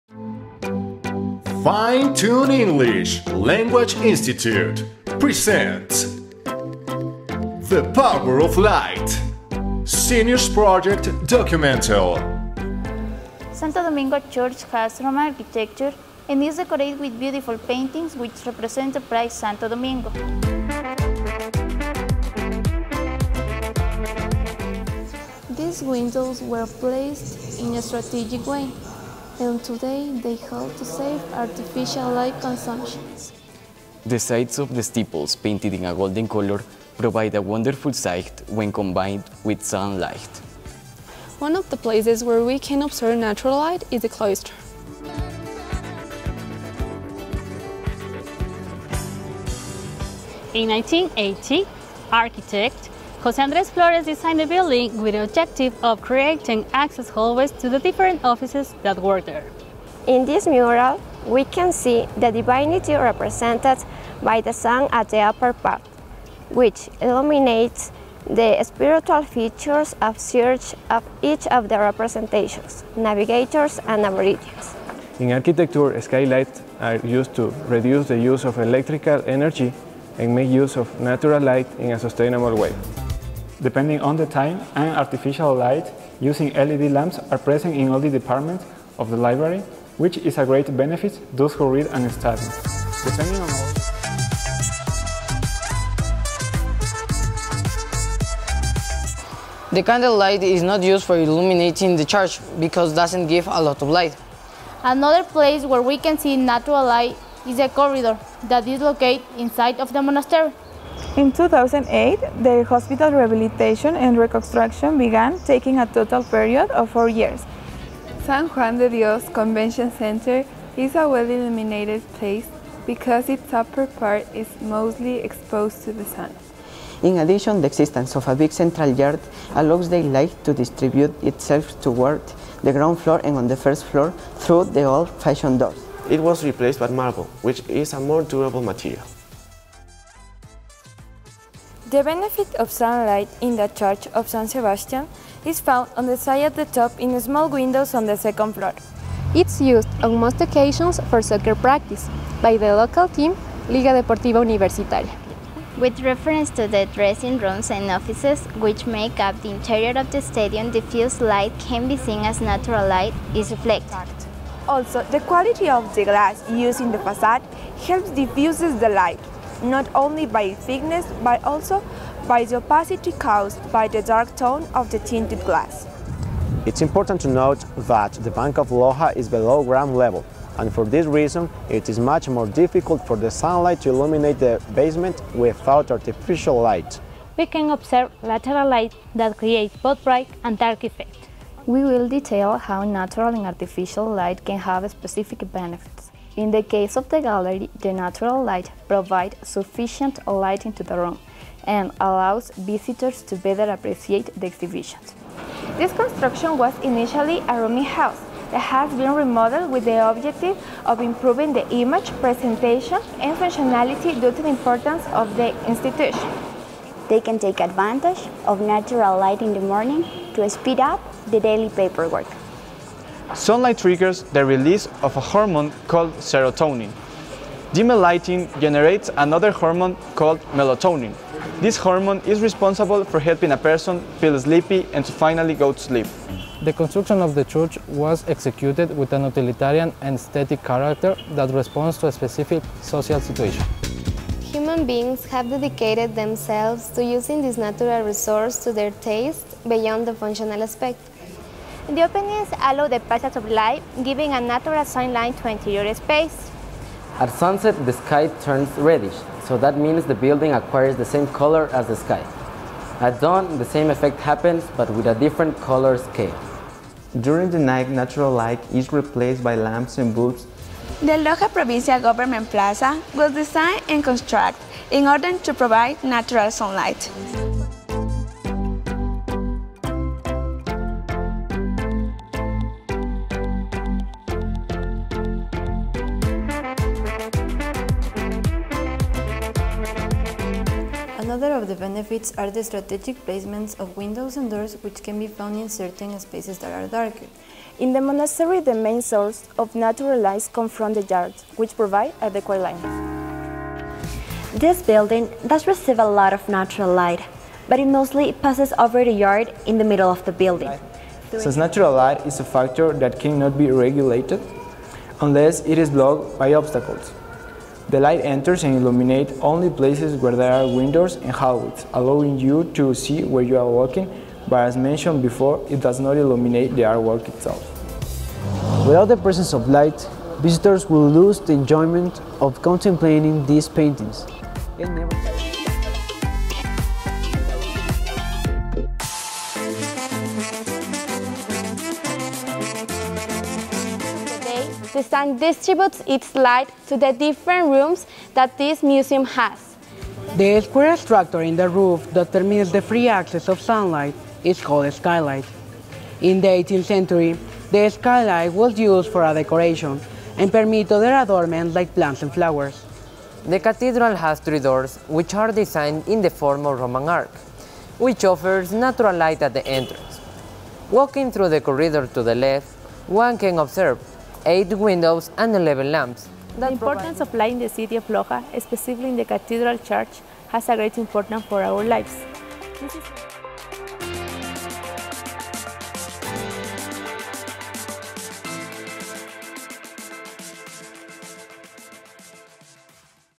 Short Film - Power of light (Documentary)
Students from Fine Tuned English presents the documentary ¨The power of light¨ , where they analyze how light affects different emblematic places from Loja - Ecuador. In this video, we present the complete Short Film.